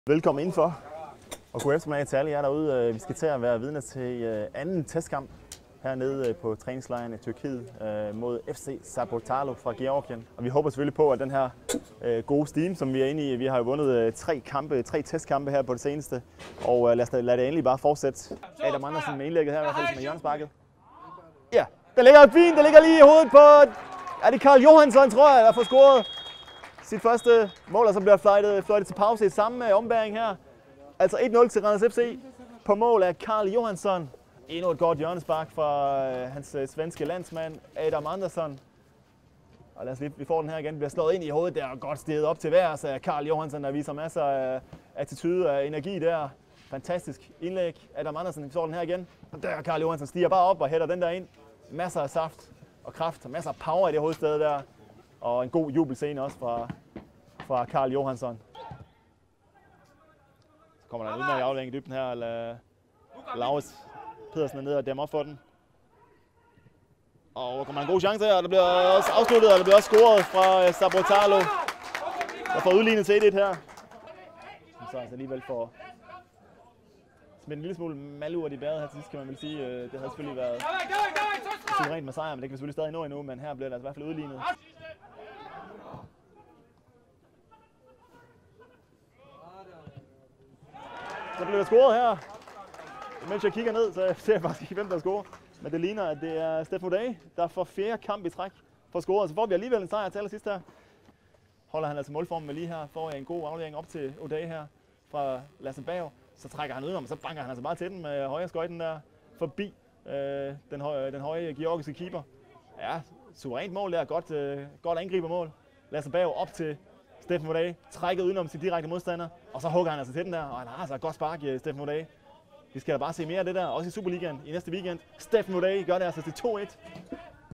Højdepunkter: Randers FC - FC Saburtalo Tbilisi 2-1
Med en uge til forårspremieren i 3F Superligaen skulle Randers FC søndag finpudse formen med to testkampe under træningslejren i Tyrkiet.

I den første kamp besejrede Randers FC georgiske FC Saburtalo Tbilisi med 2-1 👏🏼💙

Carl Johansson scorede sit første mål i Randers FC-trøjen, da han flot steg til vejrs og headede bolden i mål til 1-0, mens Stephen Odey igen blev matchvinder med sin scoring til 2-1, da han scorede for fjerde kamp i træk ⚽️🔥